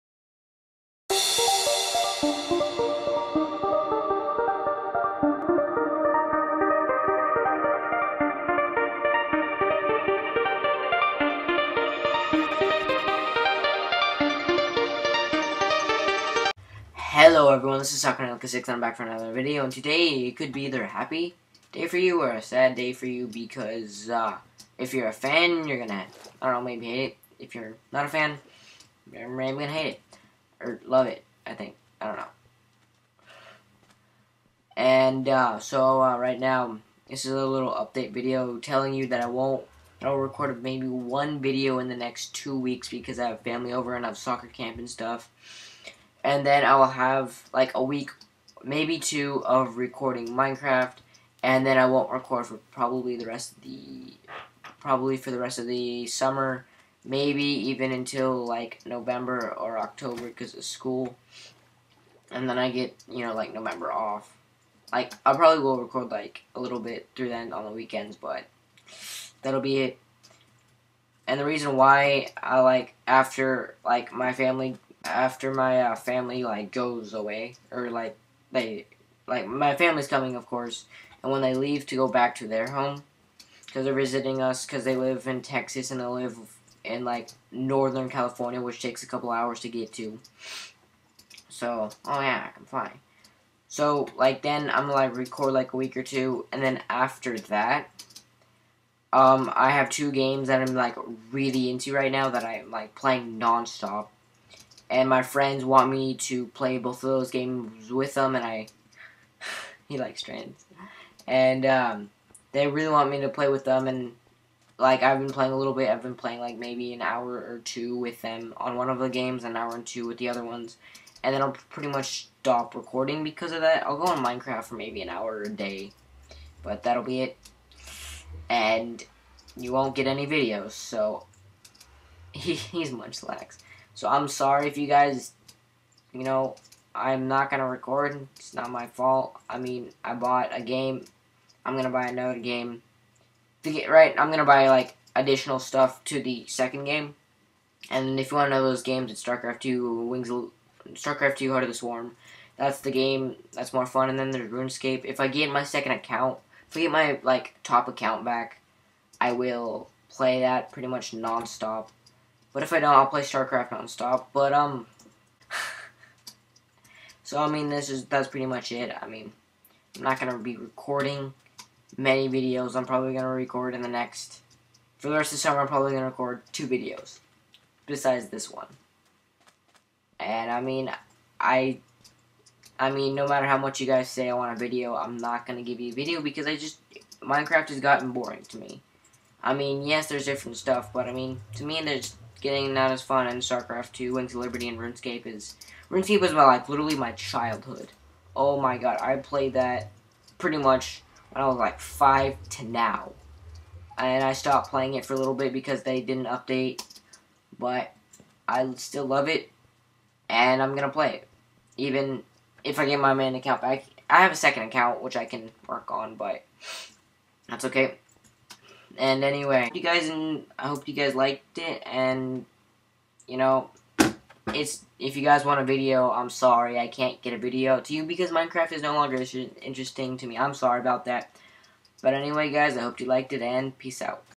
Minecraft-Huge update video
Hello everyone i am back for another video and i have an update for you!  So i wont record in 2 weeks and then record 1-2 weeks of minecraft and take like a 5 month break from recording.  I have taking that break because of Starcraft 2 Heart of The Swarm and Runescape!  Well i hope you guys can understand!